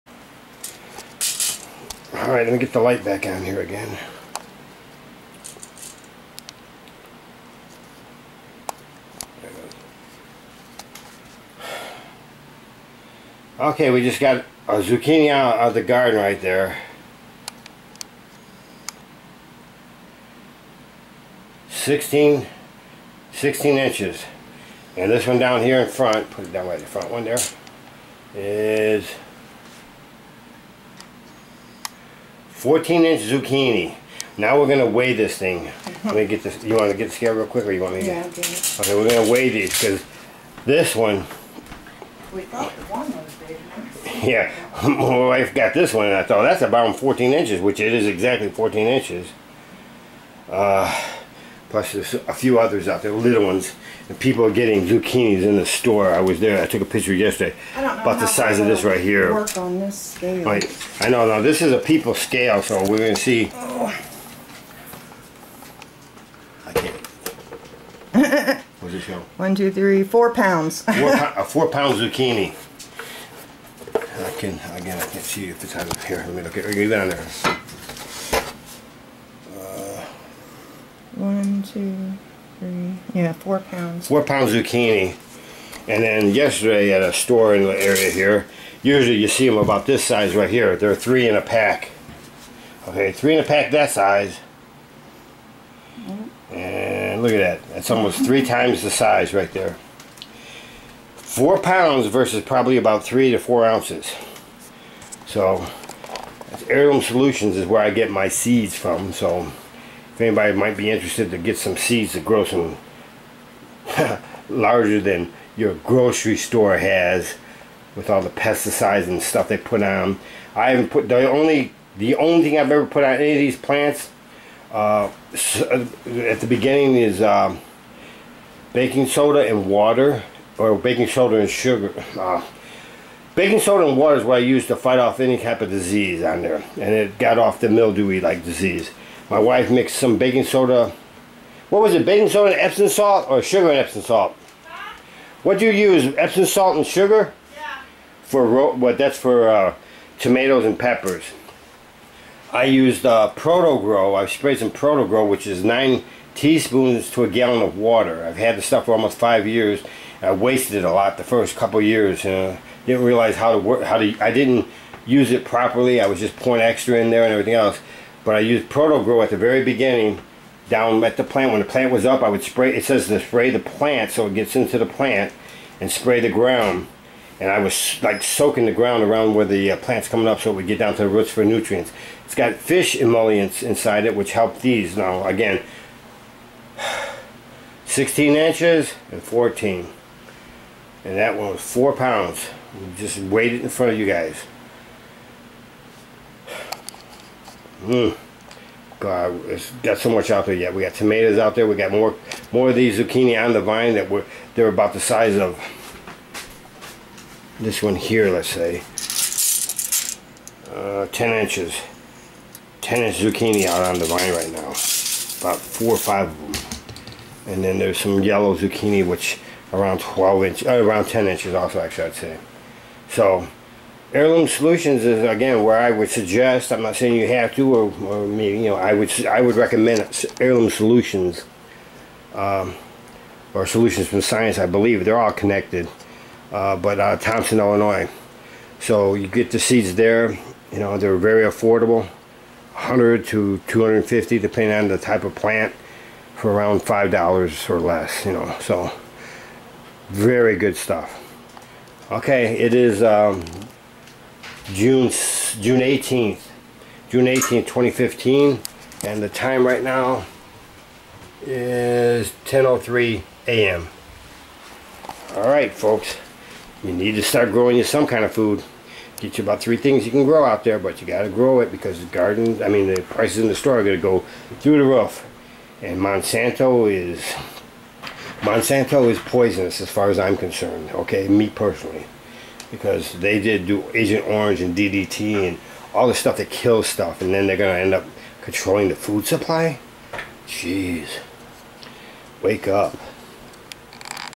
2015 06 18 BIG'N 16 inch
Big ol'  16" Zucchini  , sorry  the lighting  is dark,our ceiling light chain "broke" and the light  was from the cam.Wife ..."with" a little  help  from  me have been  CANNING and "PICKLING..THAT...lol  and daily  we have   PLENTY ! We have about 26 pepper plants, 80% of which are "BELL-PEPPERS", Green and Orange.